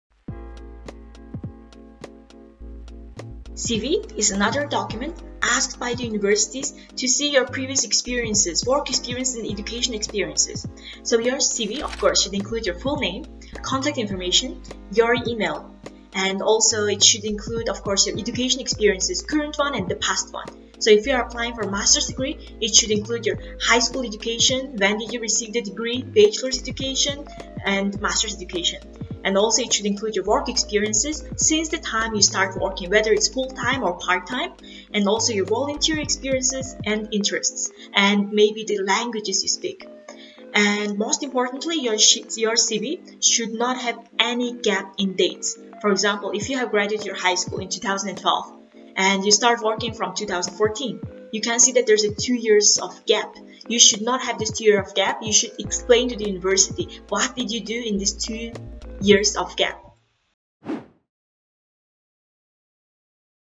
How to Prepare Your CV to Apply to Chinese Universities
The CV (Curriculum Vitae) is an important document among others required for students to apply to Chinese Universities, and Dulguun helps you make sure you don't miss out on the essentials.